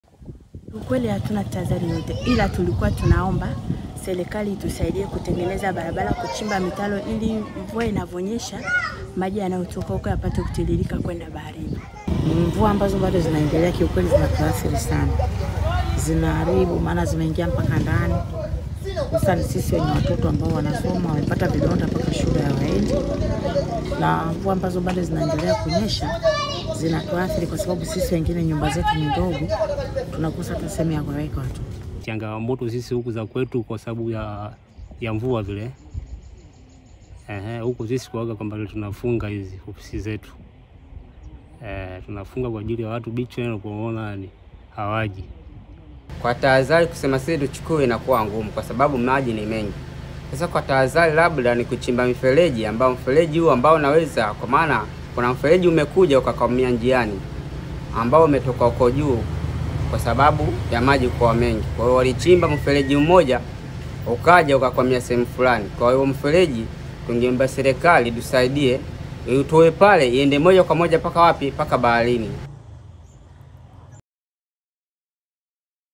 Athari za mvua Dar es Salaam
Kutokana na mvua zinazoendelea kunyesha nchini Tanzania na kwingineko Afrika mashariki wakazi wa ununio jijini Dar es salaam wameendelea kuiomba serikali yao kufanya maboresho ya miundombini hasa mitaro ya kupitisha maji machafu ili maji ya mvua yasituame kwenye makazi yao

Mamlaka ya hali ya hewa nchini Tanzania TMA imeendelea kusisitiza juu ya uwepo wa mvua  na kuwataka watu wote kuchukua tahadhari stahiki

🎥: eagansalla_gifted_sounds_